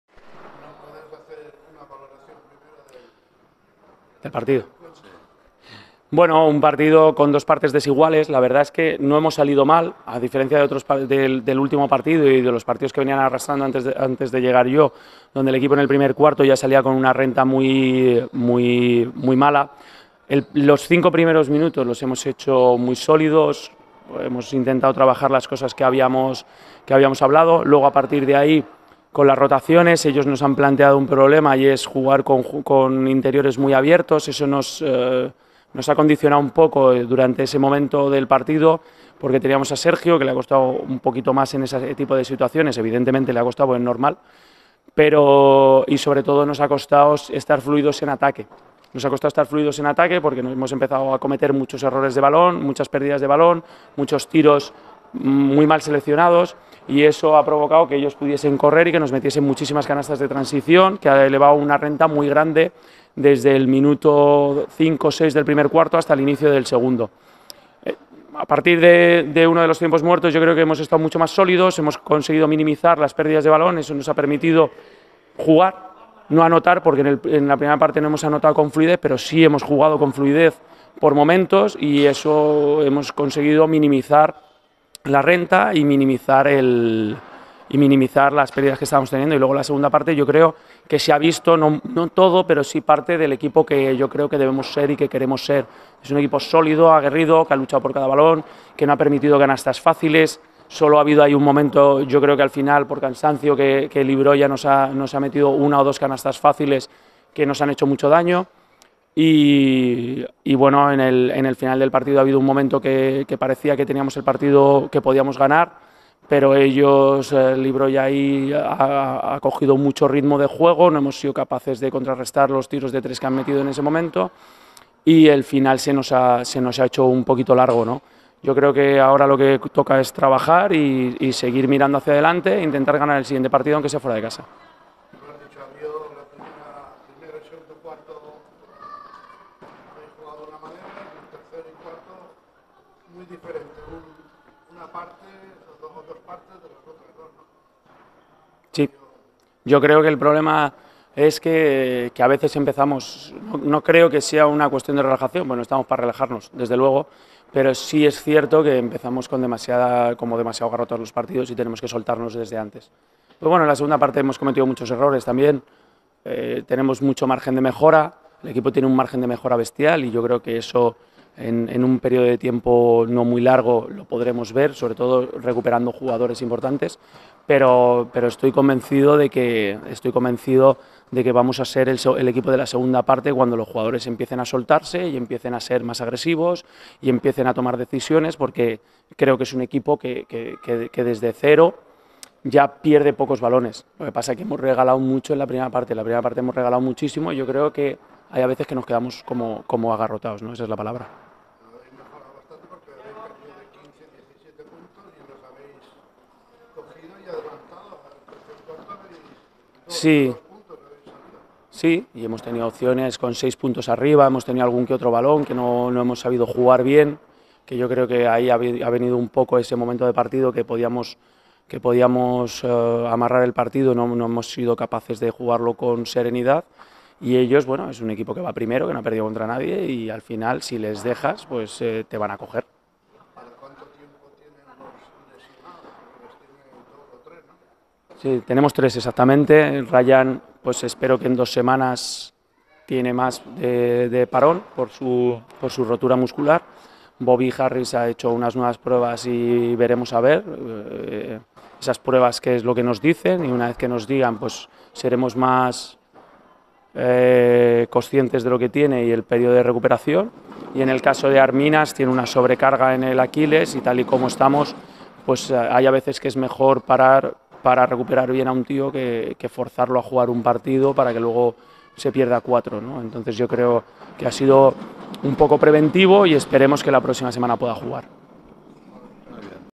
Declaraciones de Sergio Jiménez entrenador del Palmer Basket Mallorca
Después del partido, Palmer Basket Mallorca – Maderas Sorli CB Benicarló